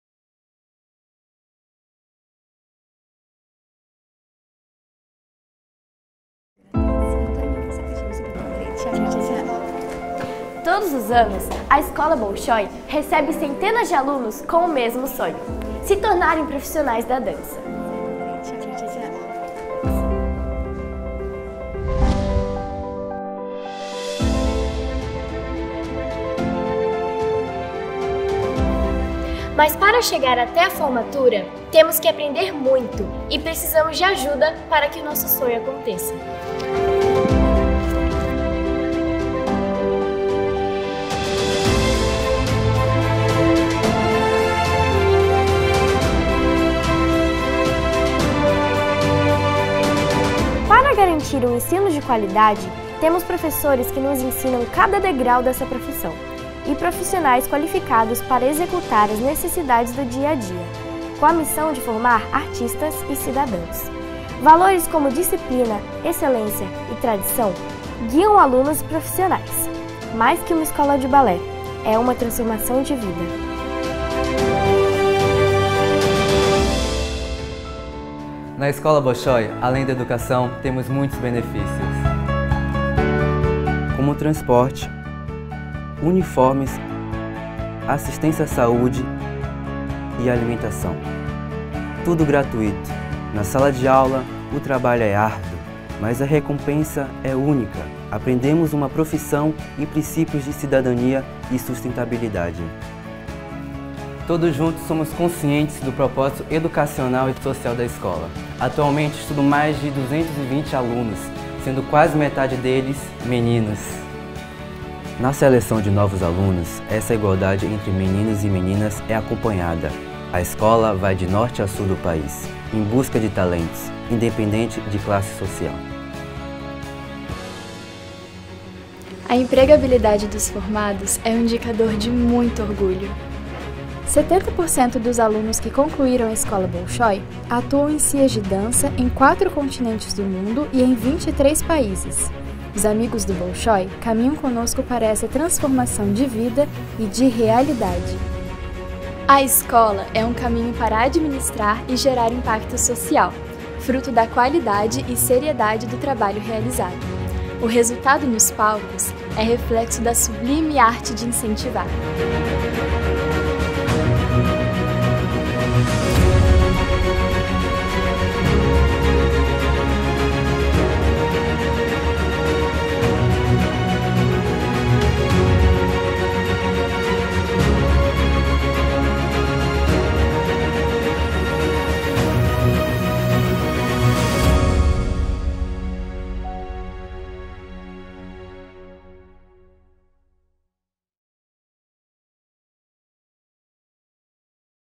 A Rotina dos bailarinos da Escola Bolshoi Brasil
Neste vídeo mostramos o dia a dia dos alunos da Escola Bolshoi e a missão de formar artistas e cidadãos. A Escola do Teatro Bolshoi no Brasil está há 22 anos disseminando a cultura e a arte pelo Brasil e se orgulha pelos 70% de alunos formados que atuam na área da dança, nos palcos do Brasil e do exterior, fomentando o acesso à arte e levando o nome do nosso país, estado e cidade para o mundo.
Além de ensino gratuito, os alunos recebem benefícios como alimentação, transporte, uniformes, figurinos, assistência social, orientação pedagógica, assistência odontológica preventiva, atendimento fisioterápico, nutricional e assistência médica de emergência/urgência pré-hospitalar. Os alunos recebem educação, aprendem uma profissão, exercitam responsabilidade e constroem cidadania.